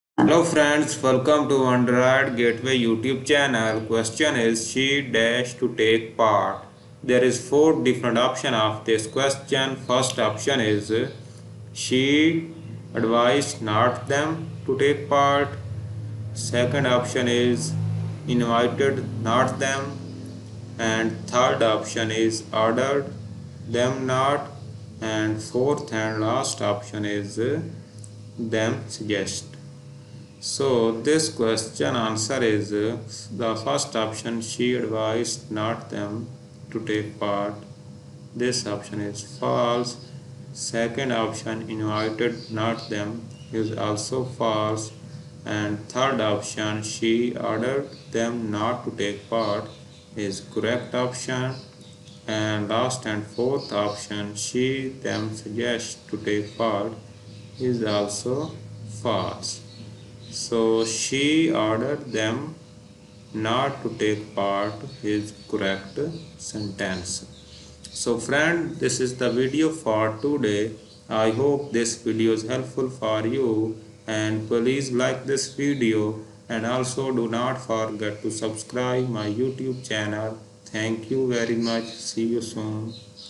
She ____ to take part. | Correct Answer!
She ____ to take part.
a) advise not them
b) invited not them
c) ordered them not
d) them suggested